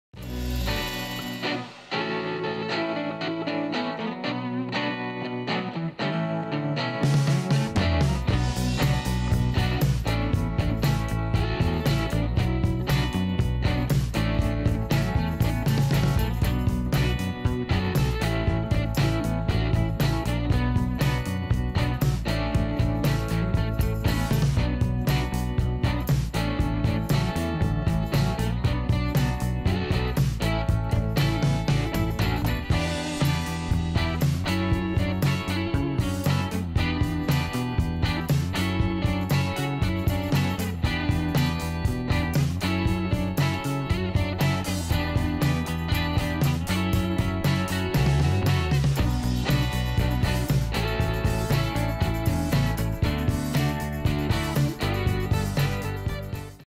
Dental Care Studios - REVIEWS - Glendale, CA - Dentist Reviews
TODAY'S REVIEW: Dental Care Studios is a highly rated dentist in Glendale, CA. This video highlights just some of the amazing feedback they have gotten from their happy clientele.

*Real reviews by real customers*